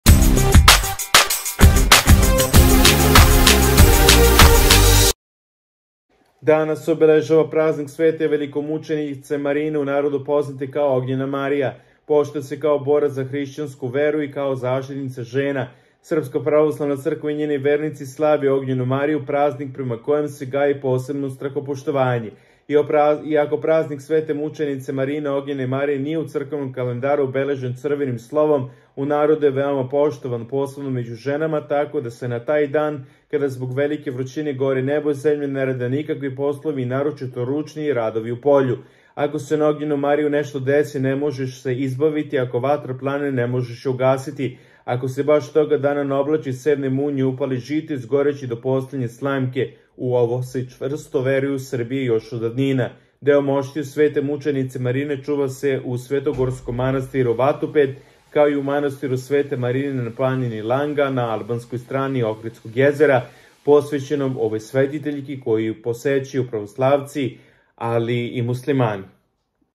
Danas je OGNJENA MARIJA! Veliki Svetac! Pazite se dobro! Evo sta ce se desiti onima koji su ZGRESILI
Sutra je OGNJENA MARIJA! Veliki Svetac! Pazite se dobro! Evo sta ce se desiti onima koji su ZGRESILI